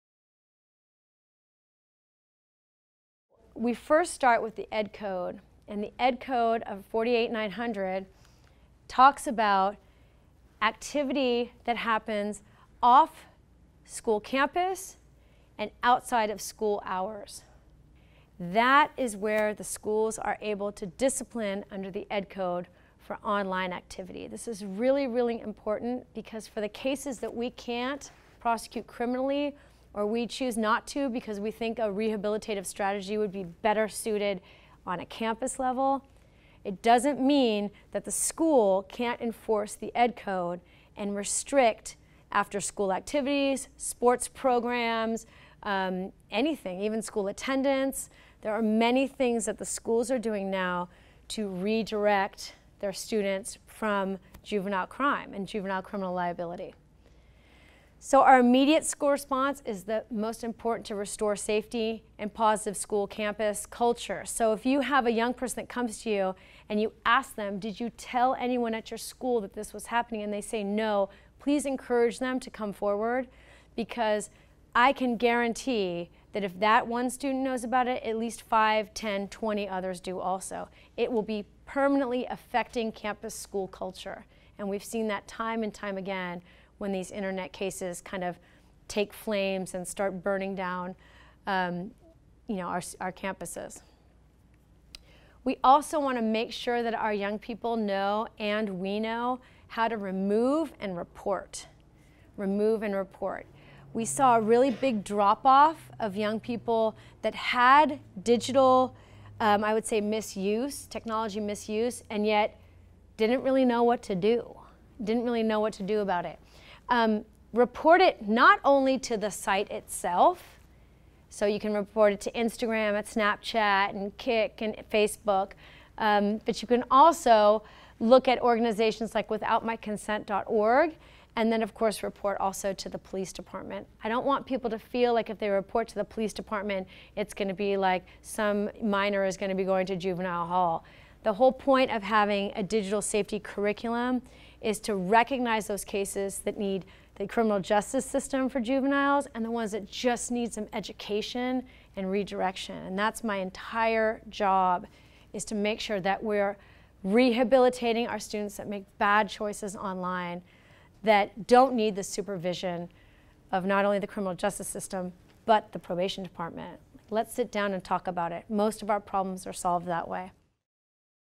Criminality, Education, & Redirection
When does online behavior become criminal or a violation of the education code? Our behavior online is subject to legal review, school review, and social review. Could you be arrested? Could you lose your scholarship or job? There are real life consequences to your online choices.